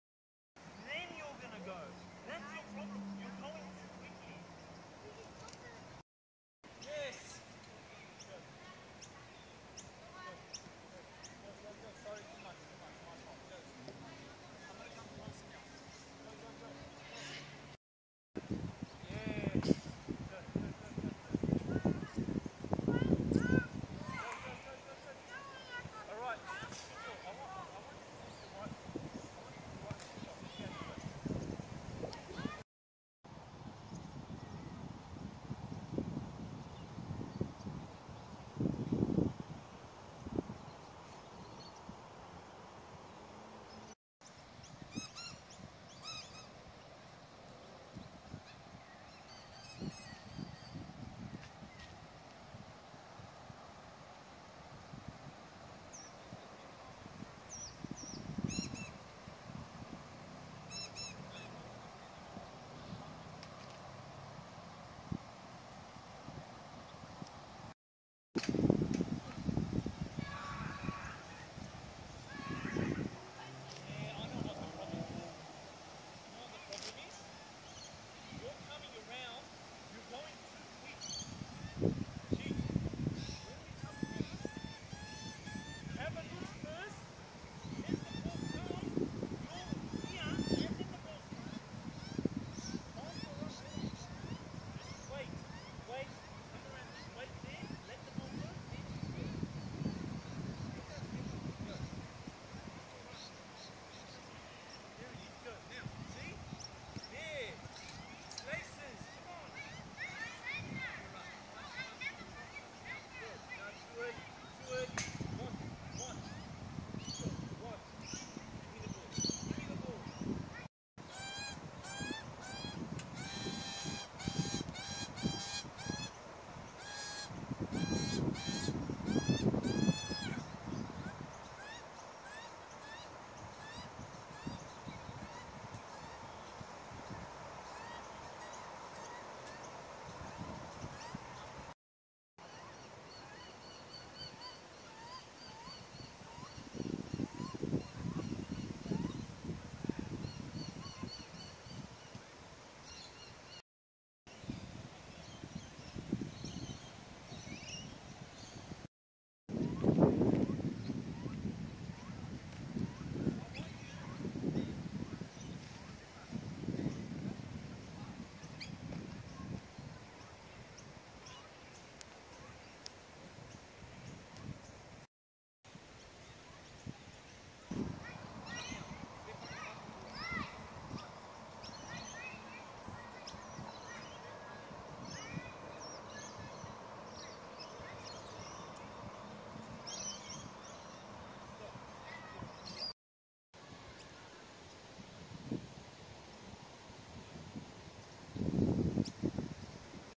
钟鹊和它们的小宝贝 Magpie and their baby
钟鹊夫妻和它们的小宝贝生活在  Upjhon Park.  它们看着孩子们在打棒球，它们在树上休息，在草地上觅食。黑头矿鸟常常在一起凑热闹。😊
A pair of magpie and their baby live in Upjohn park.  There, they boys play baseball; They rest in the tree, and forage in the grass.  Noise miner ( birds ) often join the fun together.